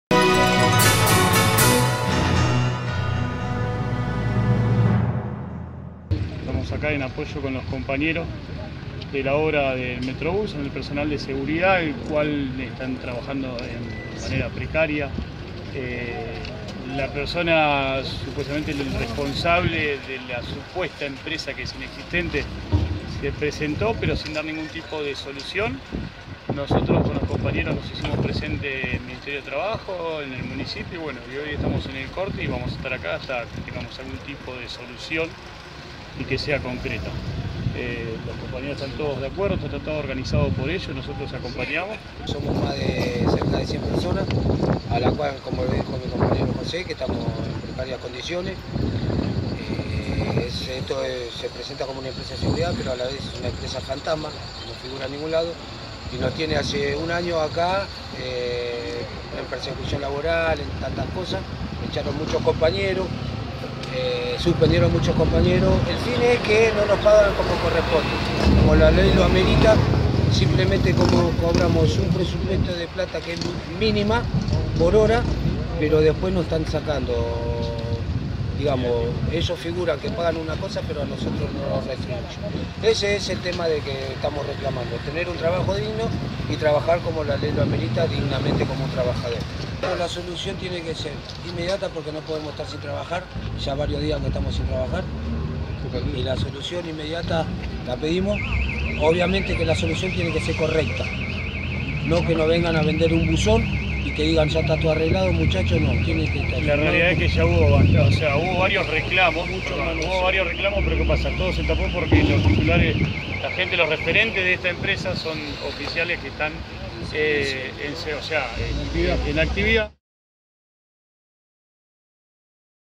Trabajadores de vigilancia del Metrobus cortaron la Ruta Nº 3
Los empleados de seguridad interrumpieron el tránsito a la altura del kilómetro 22 de la autovía nacional, en el cruce con las vías de Isidro Casanova. Piden mejores condiciones laborales y un aumento salarial.